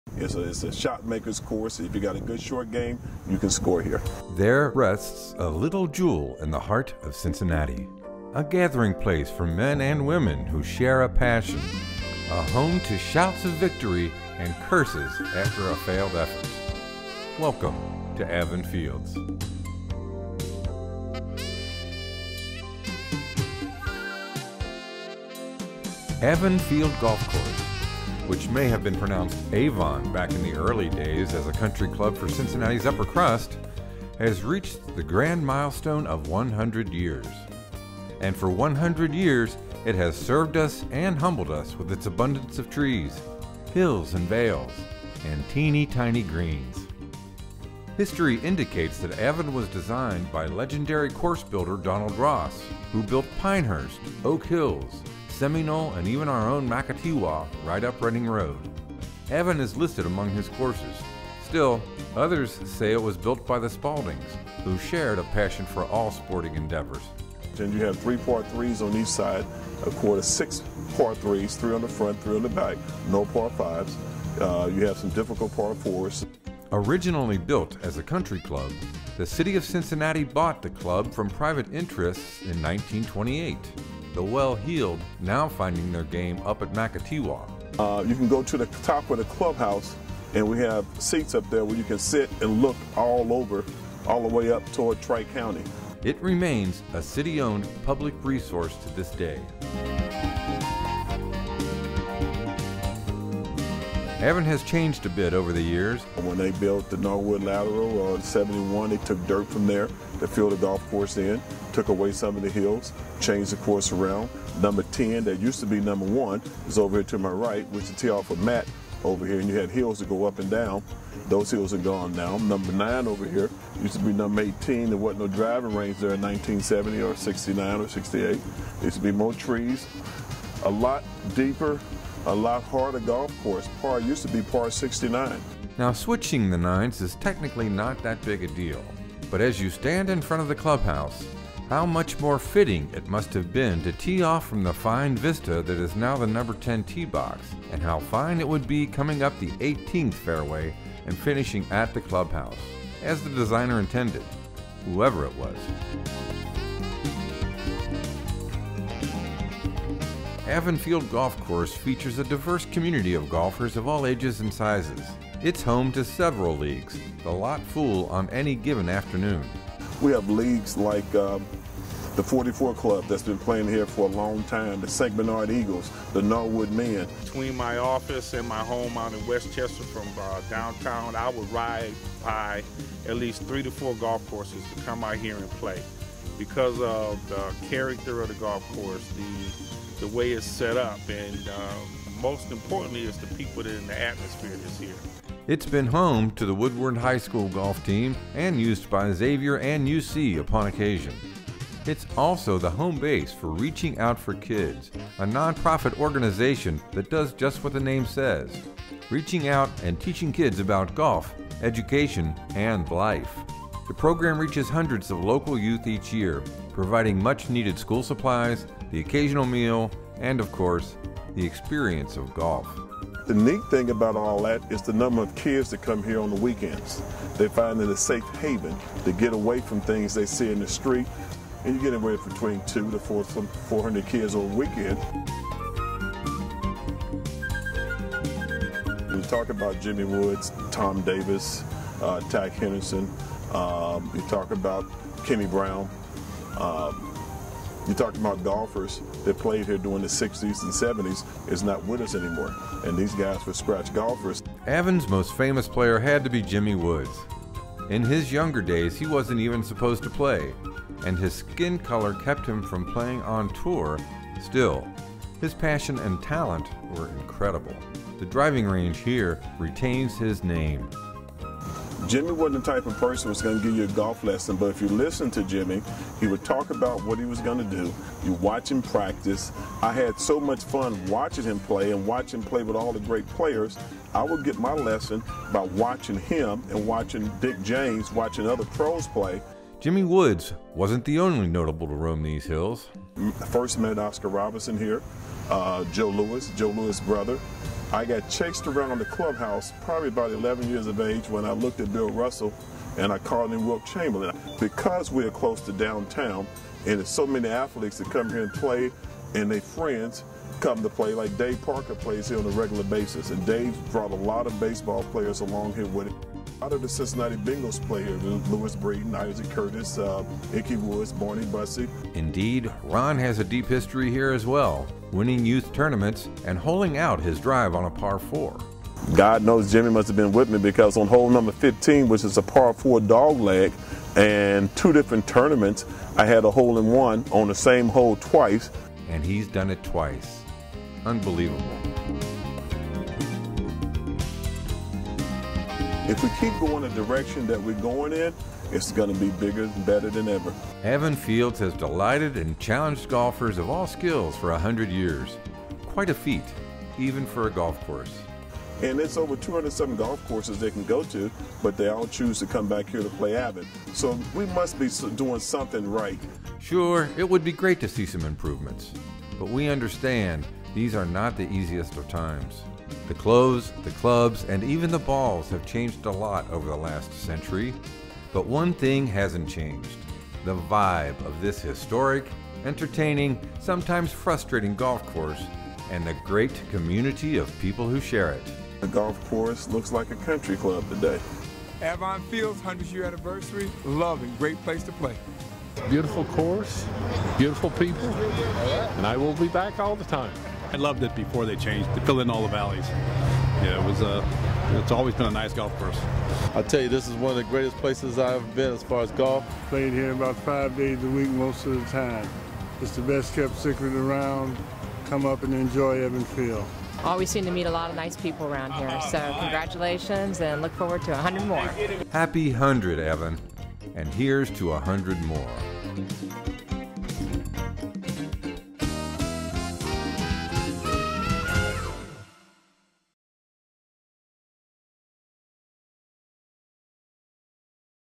Avon Fields - The First 100 Years
Avon Fields in Cincinnati Ohio, is the oldest golf course west of the Allegheny Mountains here in the U.S. It's barely 4 miles from downtown Cincinnati, and was designed by the legendary Donald Ross.